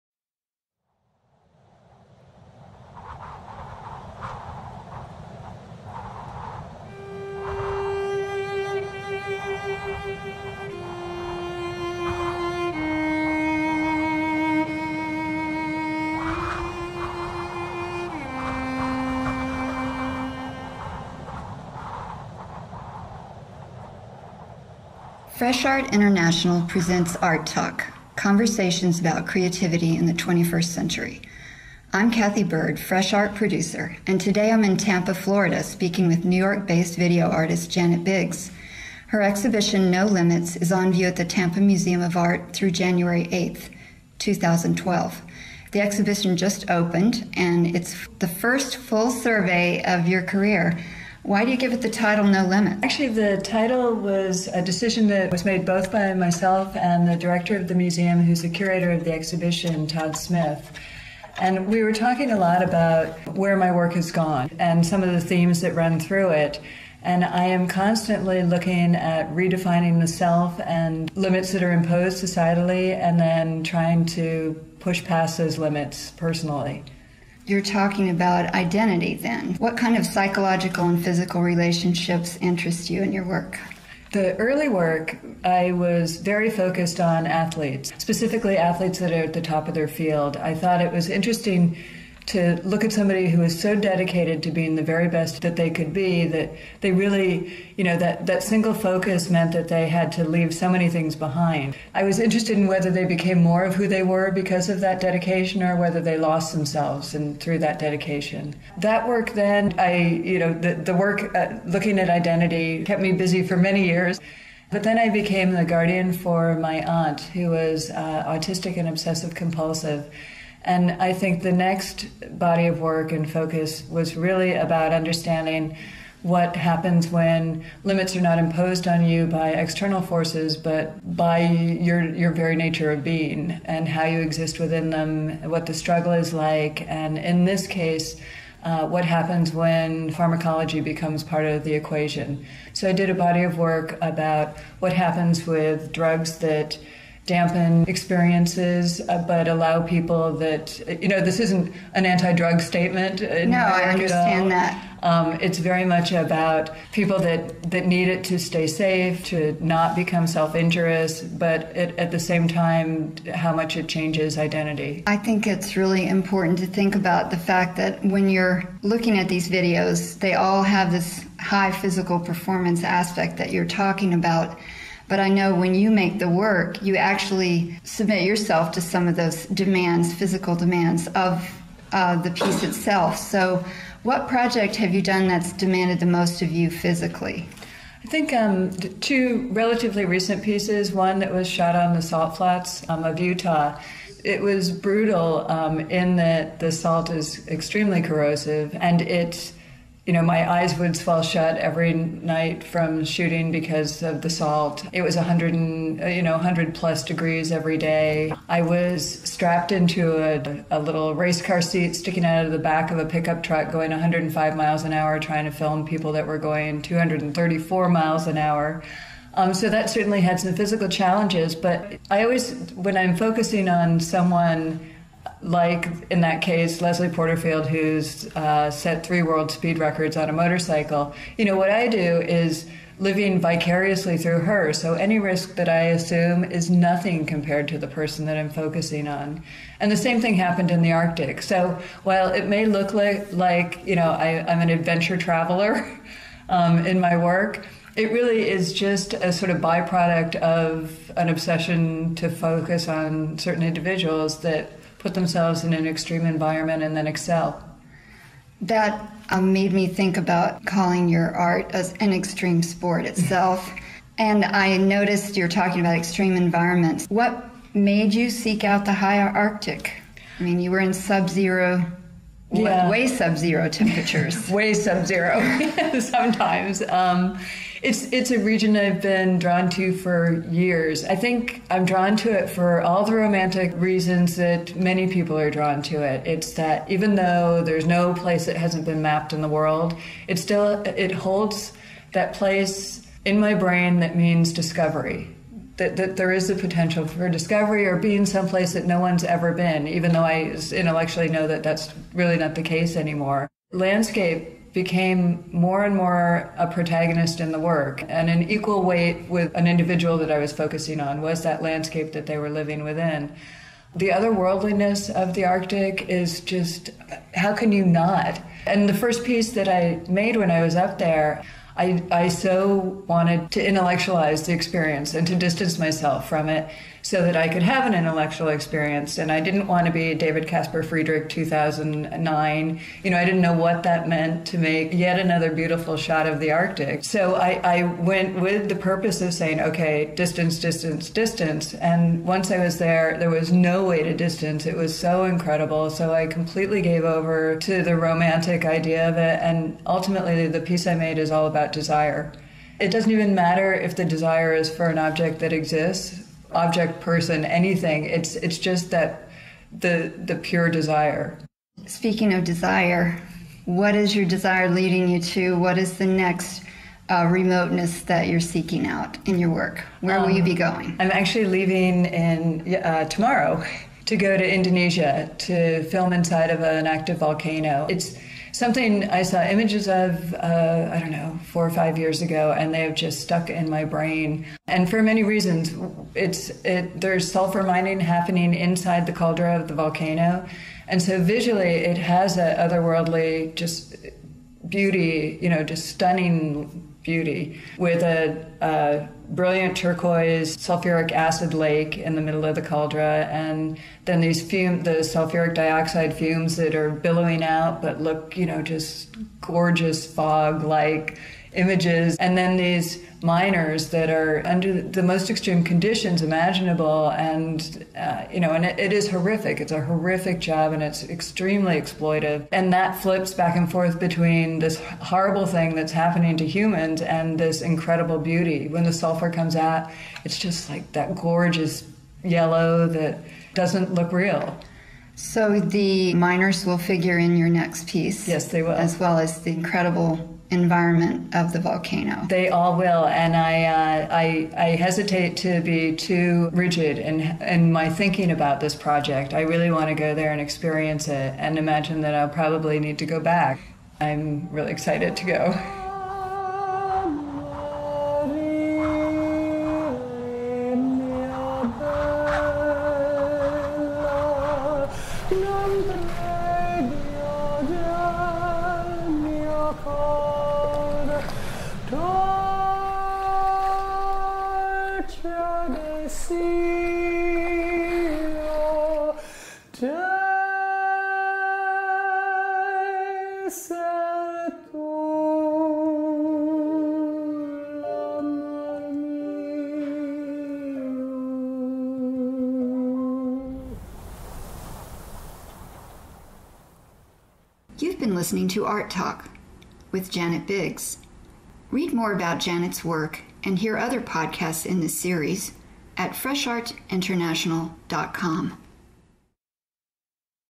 Fresh Talk: Janet Biggs
New York-based video artist Janet Biggs makes an extreme sport of her creative expression. In this conversation, Janet considers the power of desire and the physical challenges she met while producing video projects on view in No Limits, her current mid-career survey. Organized by the Tampa Museum of Art, the exhibition runs from October 8, 2011 through January 8, 2012.